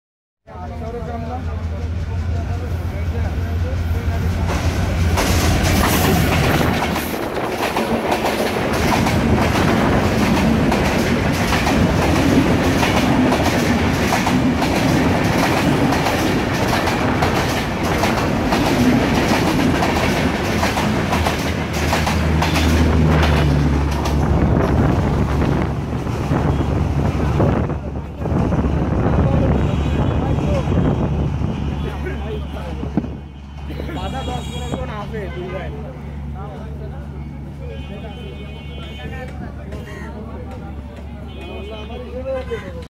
Express train bonolota || Boral bridge station through pass
সেমি নন্সটপ এক্সপ্রেস ট্রেন ঢাকা থেকে ছেড়ে এসেছে যেটা চাপাই নবাবগনজ যাবে